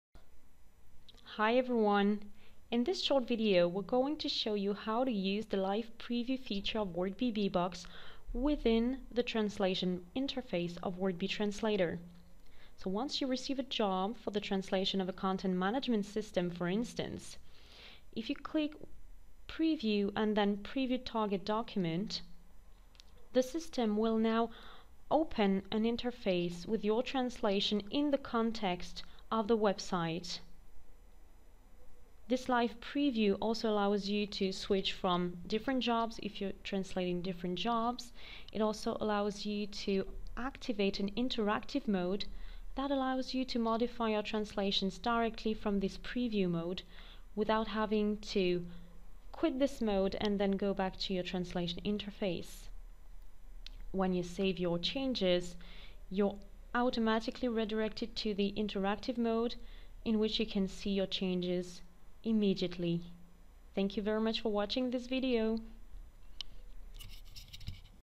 Interactive Live Preview
In this video we show you how to use the Live Preview feature of Wordbee Beebox within the translation interface of Wordbee Translator. This allows you to preview, for example, translations of a CMS in the context of your website.